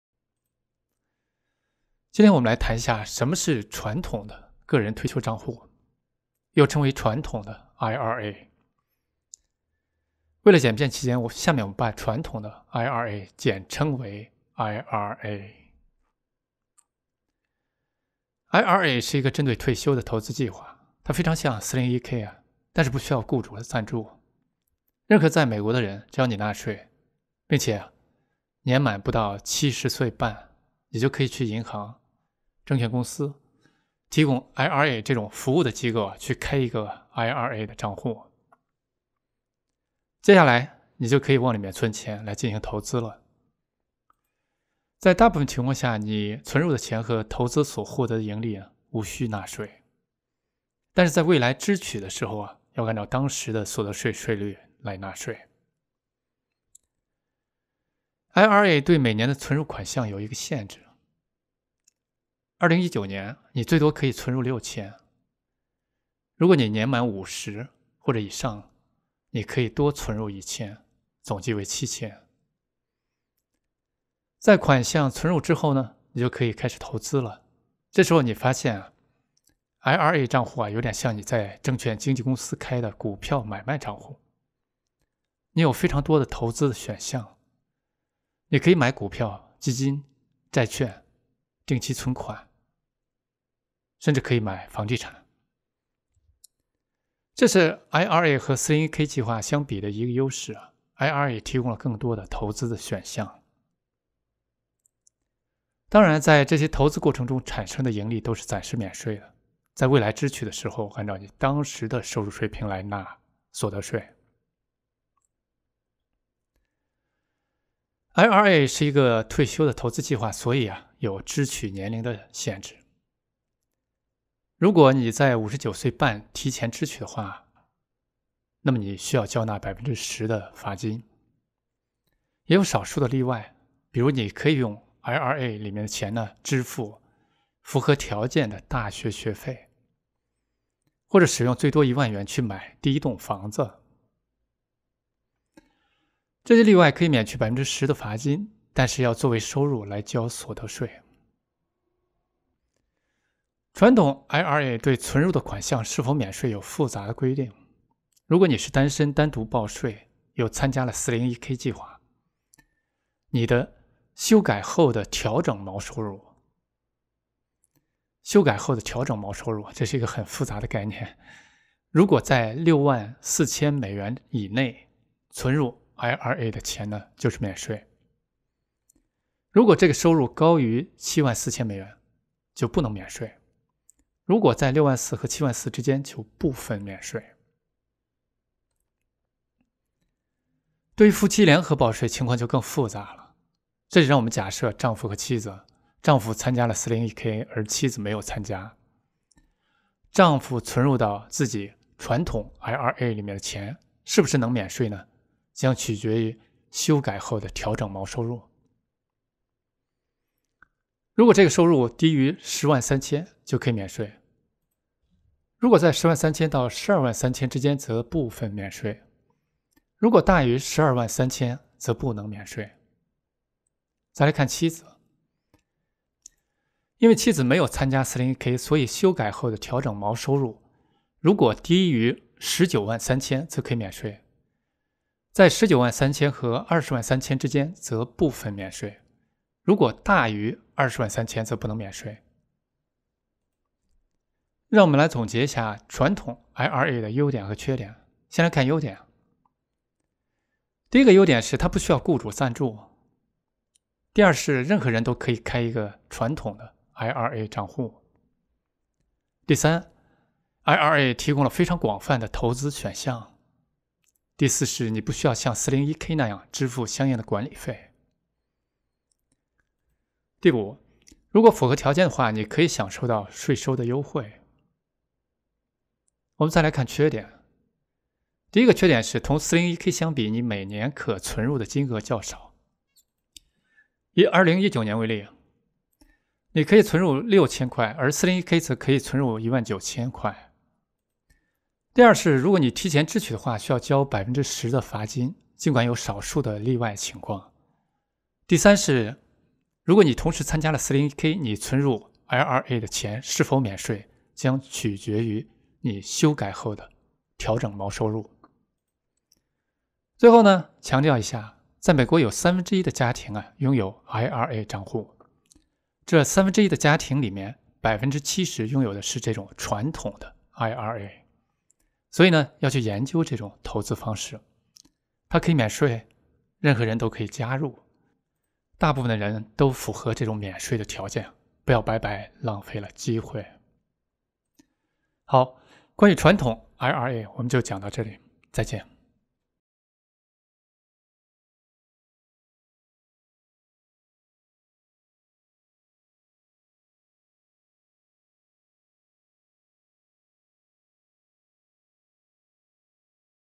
5分钟了解什么是传统个人退休账户Traditional IRA【财经思考】
个人投资理财基础知识，什么是传统个人退休计划，俗称传统IRA，或traditional IRA

传统IRA的优点
第一个优点是他不需要雇主赞助。
第二是任何人都可以开一个传统IRA账户。
第三，IRA提供非常广泛的投资选项。
第四是你不需要像401k那样支付相应的管理费。
第五是如果符合条件的话你可以享受到税收的优惠。

传统IRA的缺点
第一个缺点是同401k相比你每年可存入的金额较少。
第二是如果你提前支取的话，需要交百分之十的罚金，尽管有少数的例外情况。
第三是如果你同时参加了401k，你存入IRA的钱是否免税将取决于你的收入。